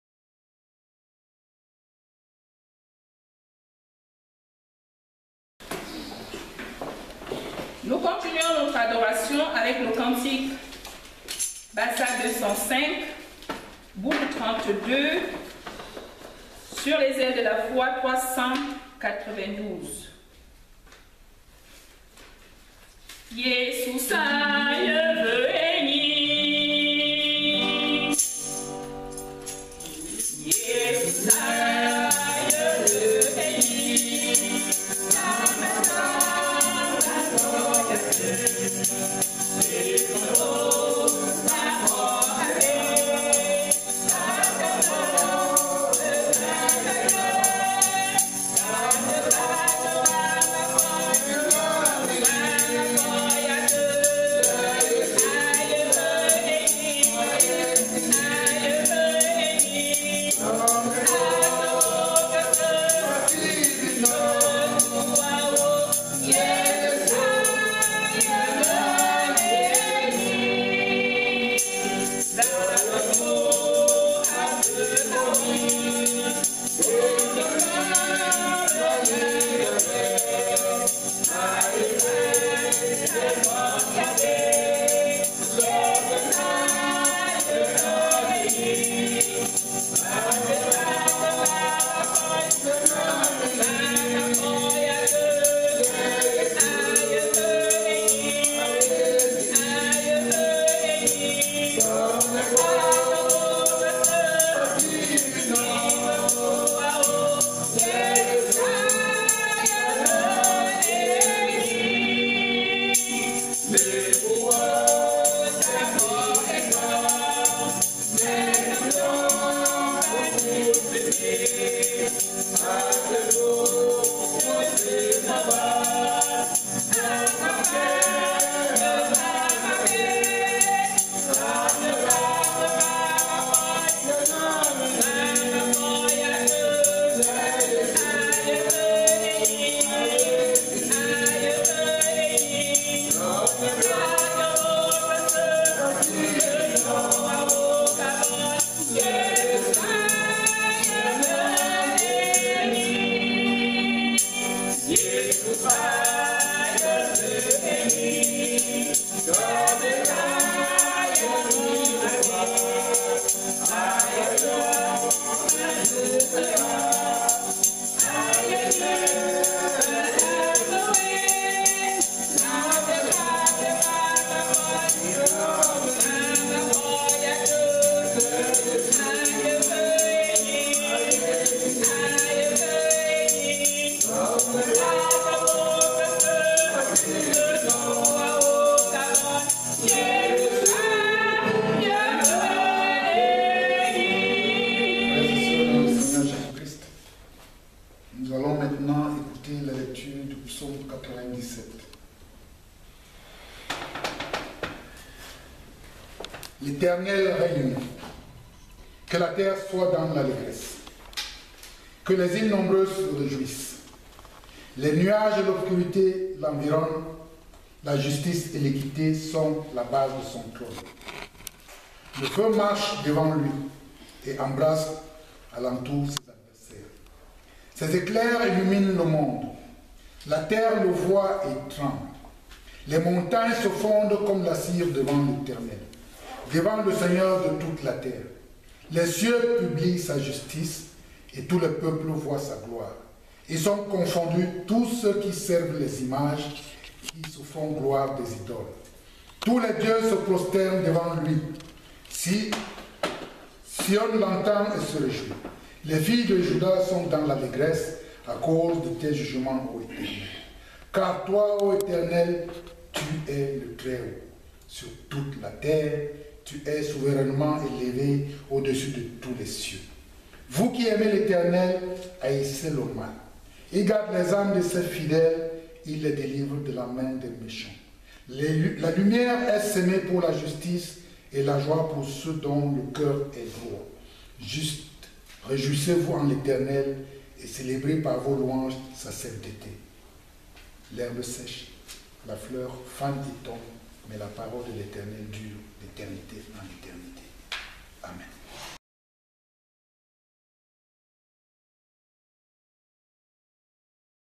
Paris Nord, Lecture du Psaume 97, Culte du 08 mars 2015
Lecture du Psaume 97 lors du culte du 8 mars 2015 à l'Eglise Presbytérienne de Paris Nord (Mission de l'EPC), faite par l'Ancien d'Eglise NYEBEL Paul